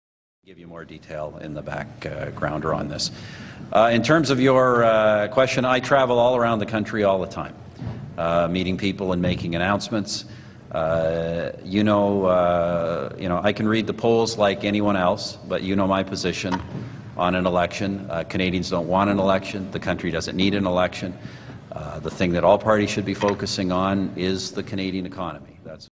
Kid faints listening to Prime Minister Harper.
Guelph, Ontario, A kid standing behind Stephen Harper faints.